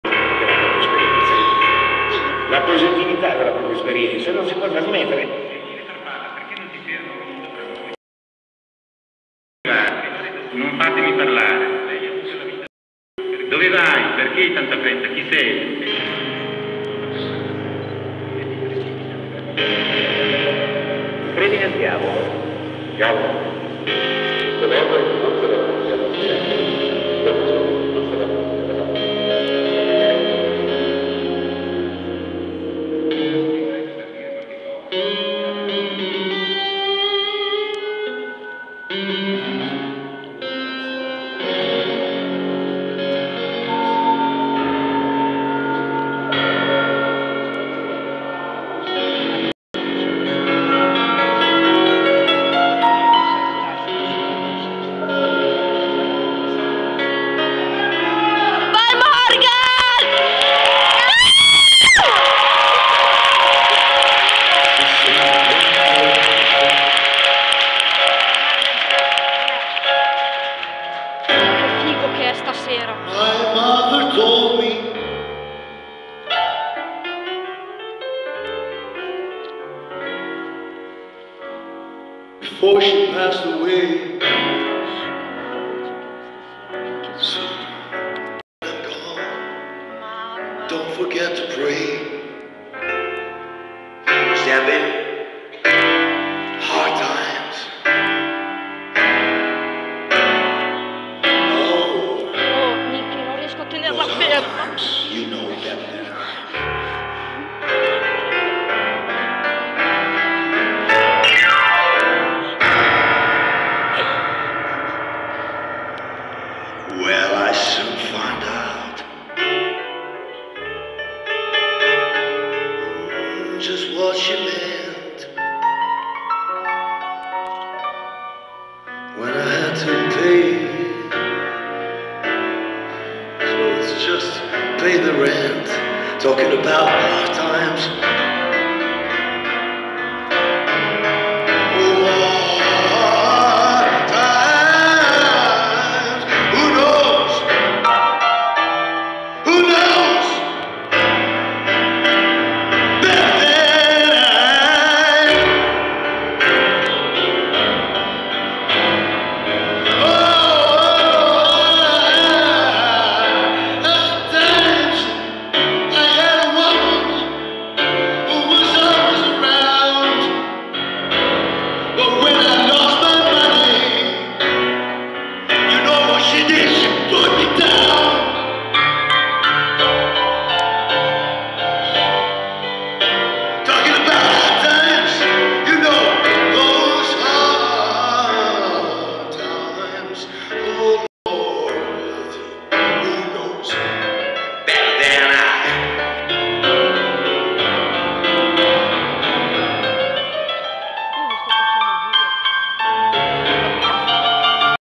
morgan a lodi 2010
mamma mia che emozione si sentono le mie grida grazie ancora niki per quella fantastica serata